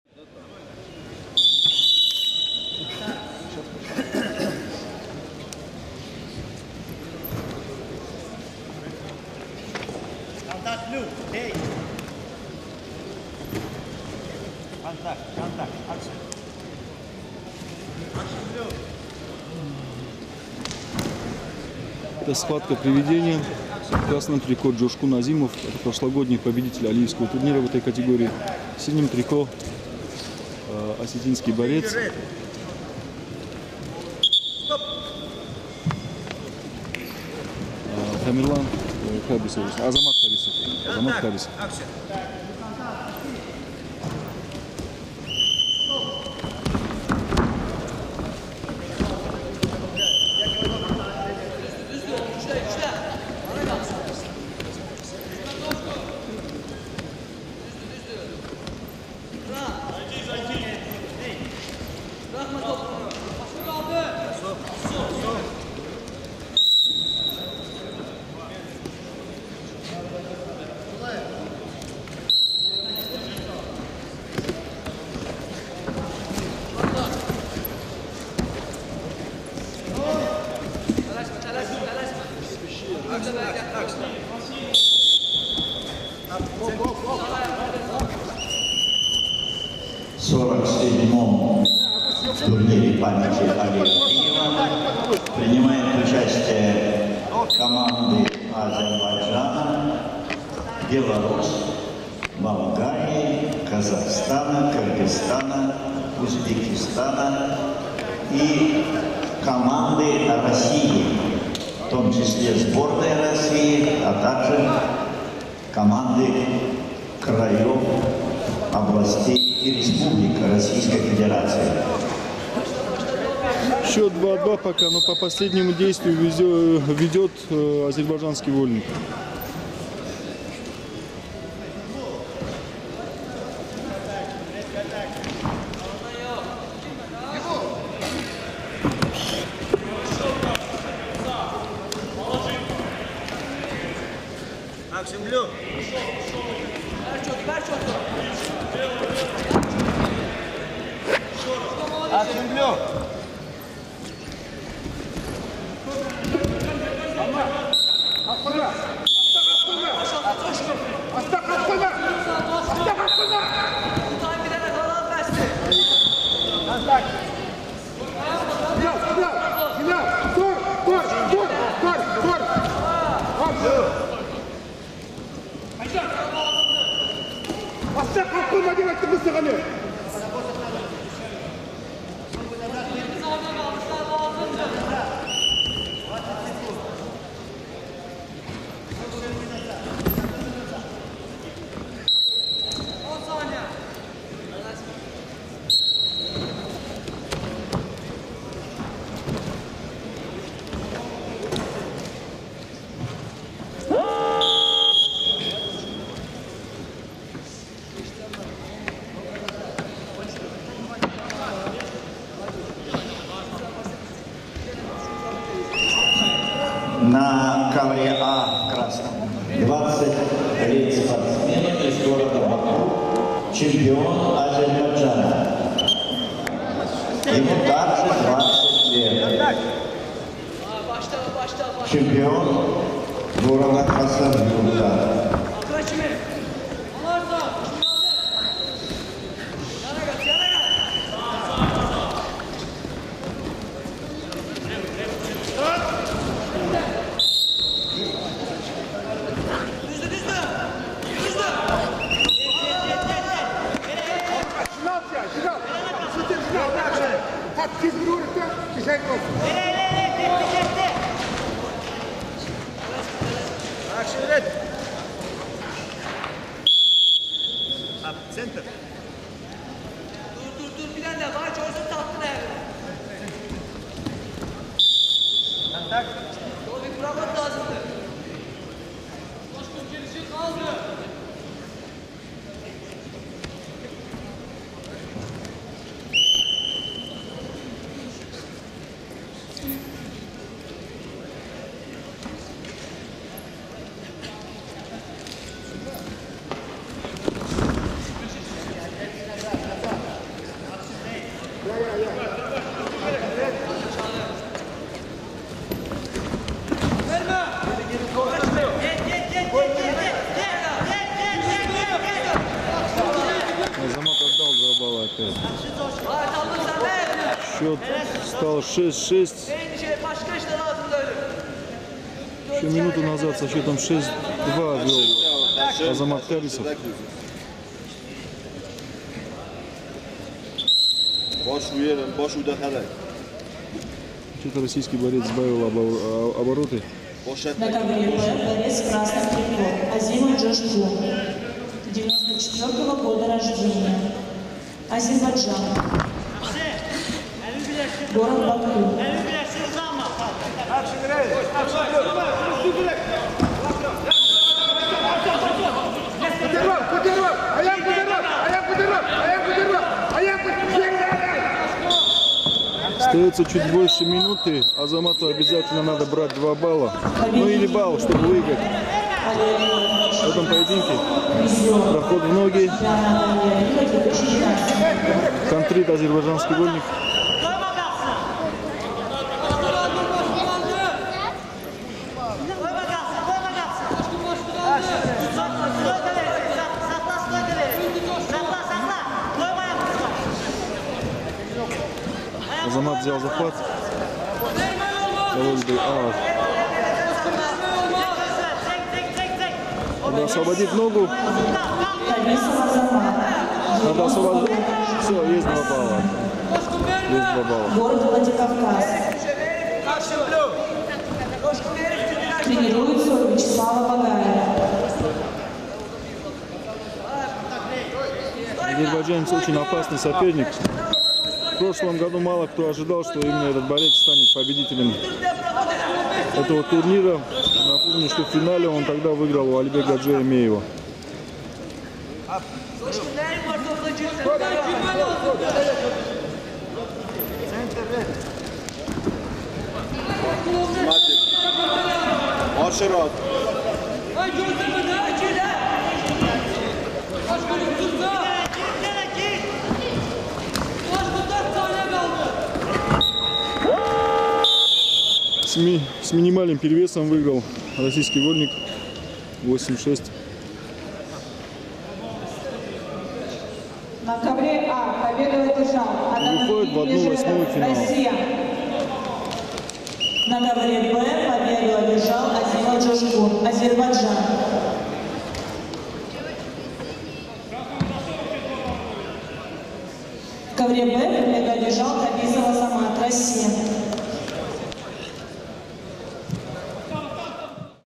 Алиевский-2016_65 кг_Азимов-Кабисов
Международный турнир памяти Али Алиева. Каспийск, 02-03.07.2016